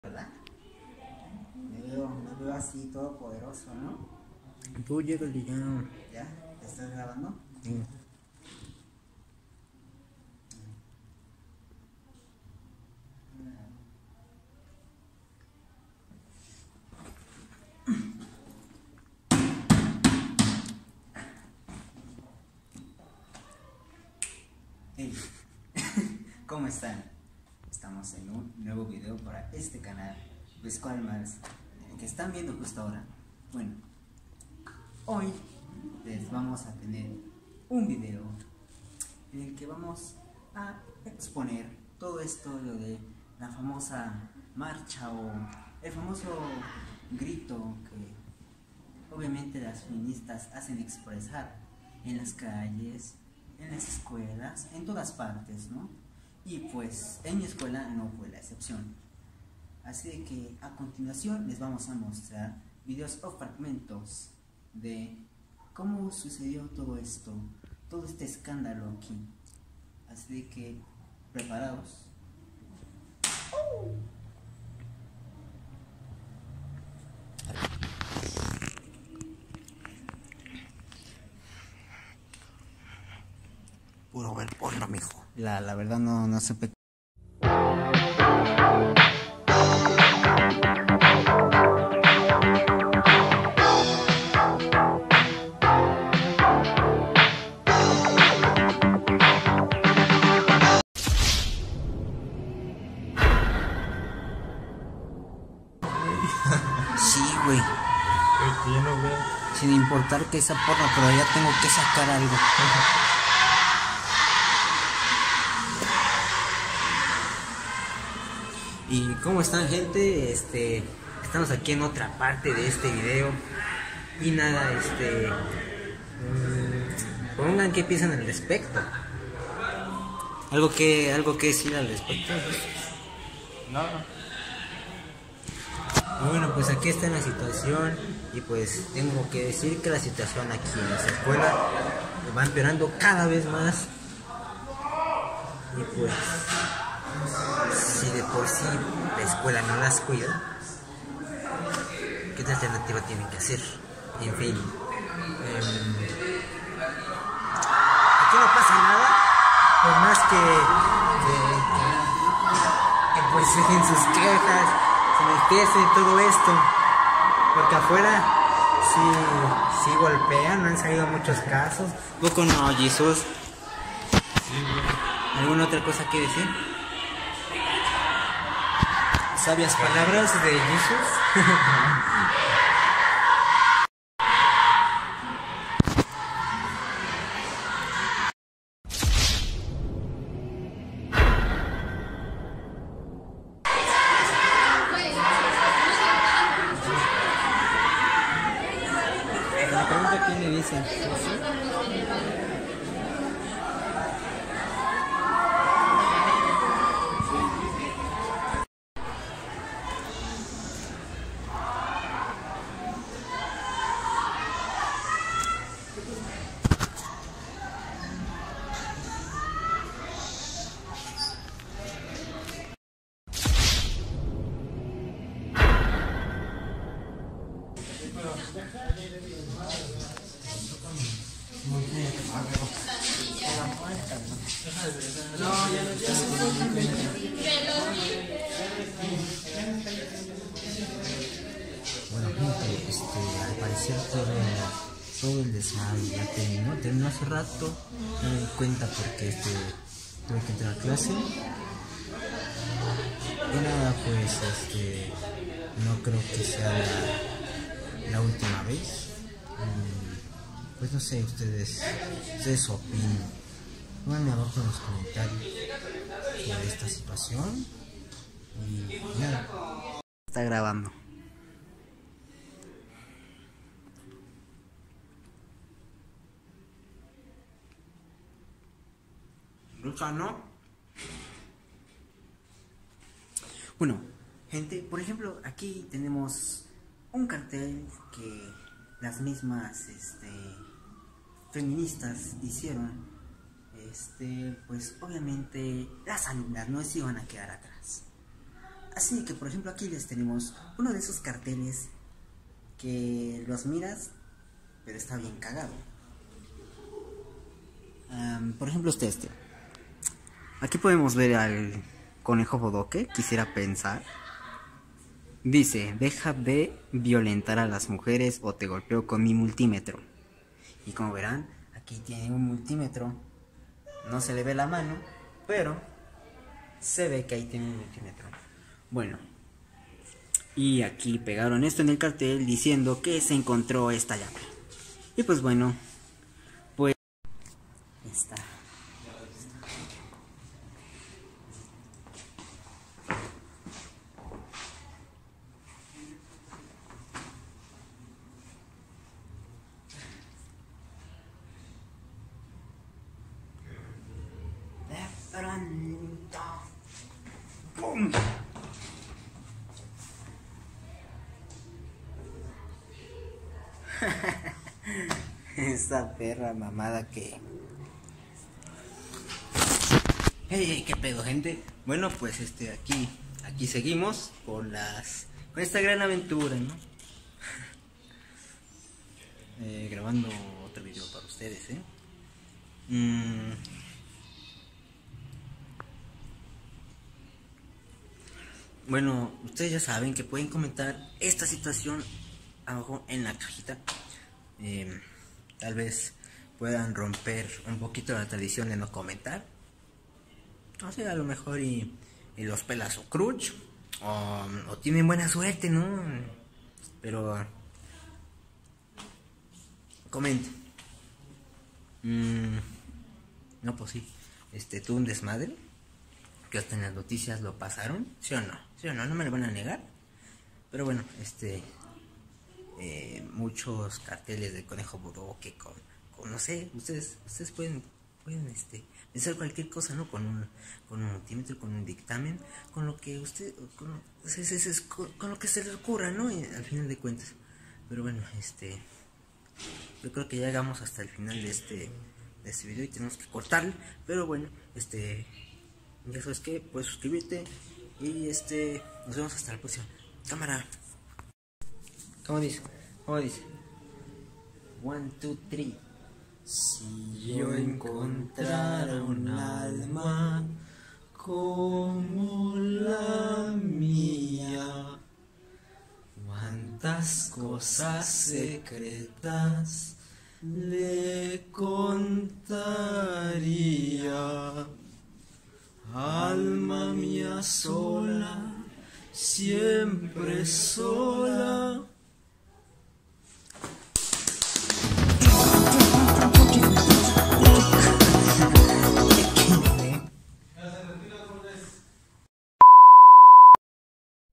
Acoso en mi escuela (parte 2) ¿cómo terminó?
Y bueno gente, aquí esta la parte final de aquel día de protesta de acoso en mi escuela espero que el videon te haya gustado y no olvides darle laik y suscribirte, ya sabes que nos vemos hasta la próxima.